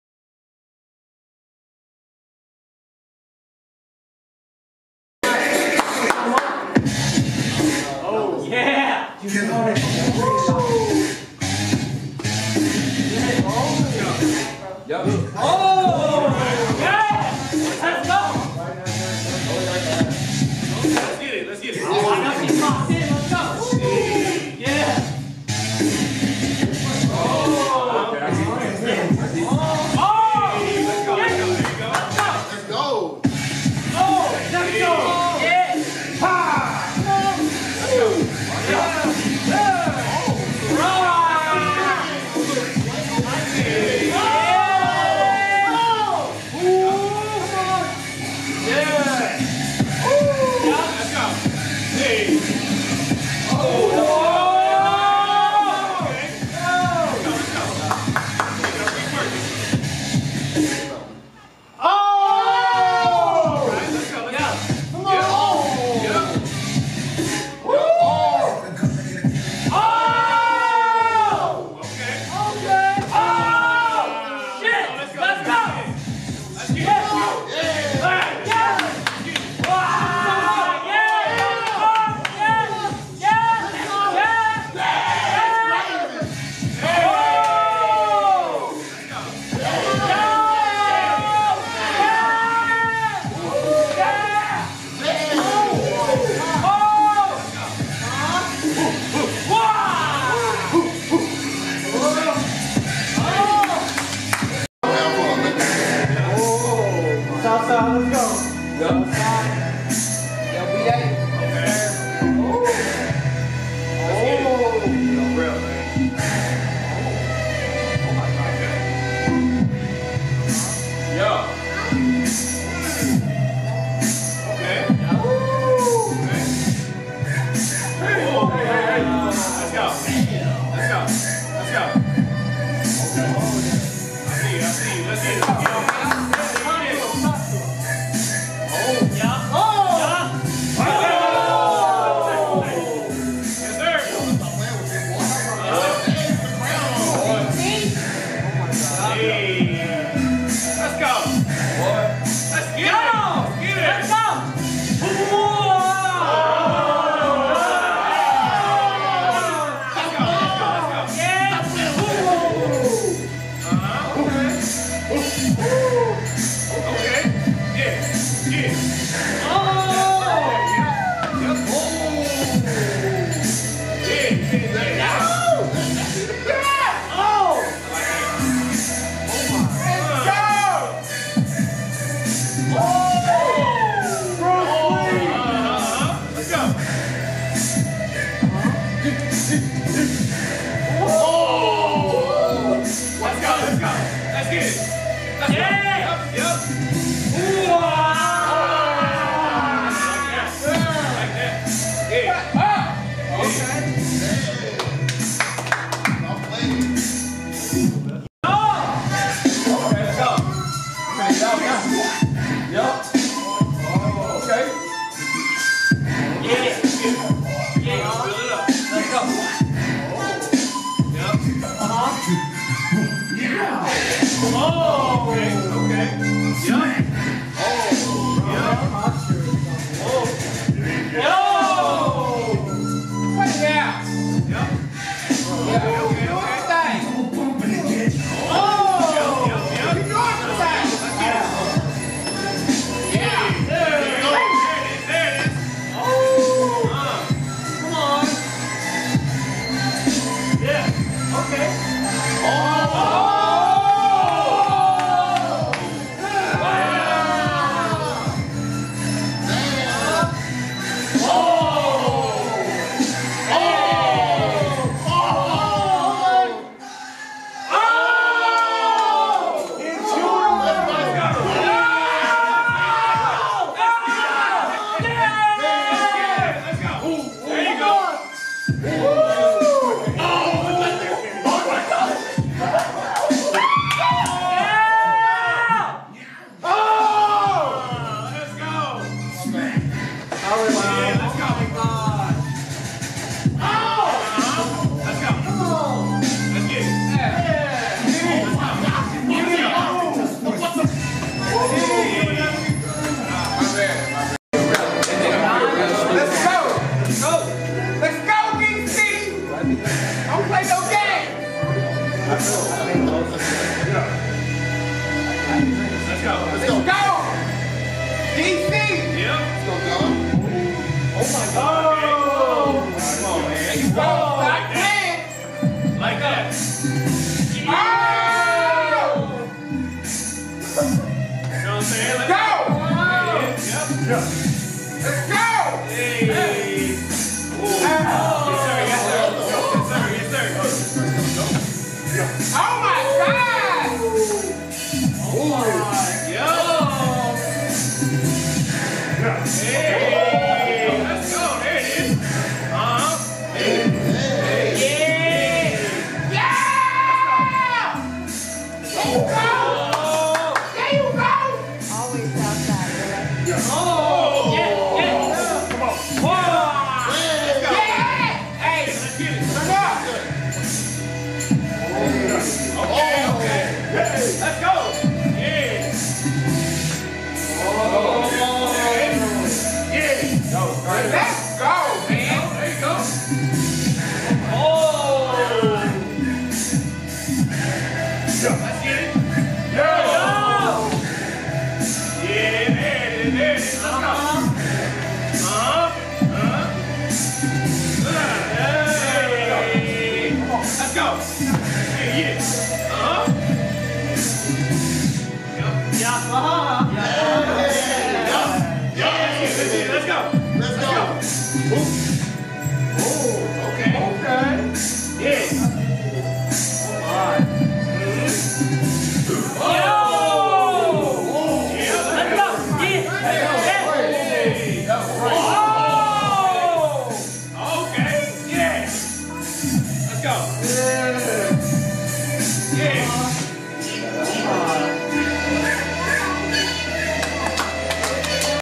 Soma Sylar vs Legacy
New Era Legacy Session

BI WEEKLY SESSION HELD IN CEDAR HILL TX.

Soma Sylar in her first battle vs Legacy